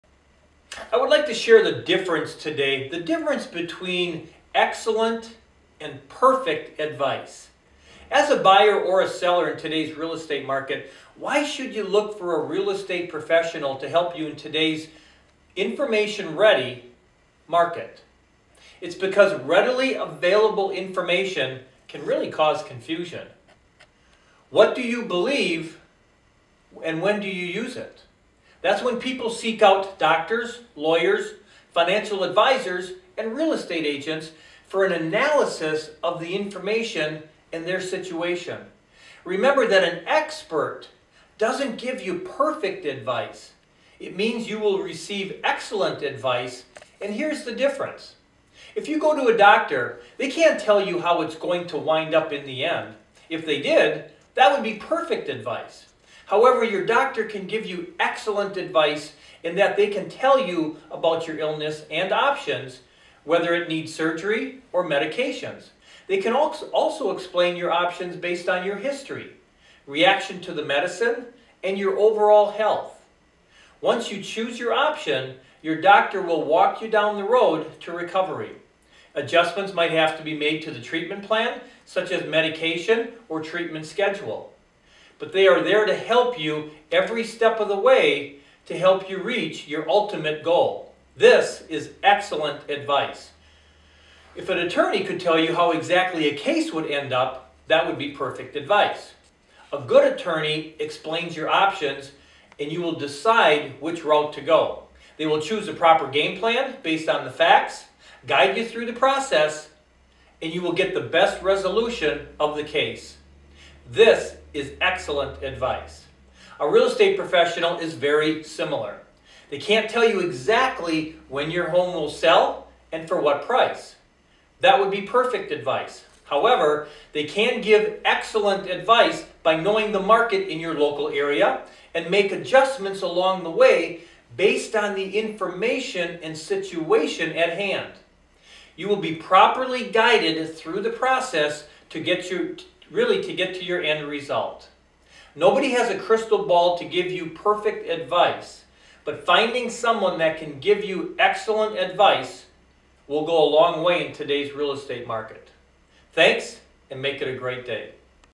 The Difference Between Excellent and Perfect Advice
Dan Ludwig, Shorewest Realtors, explains the importance of working with a real estate expert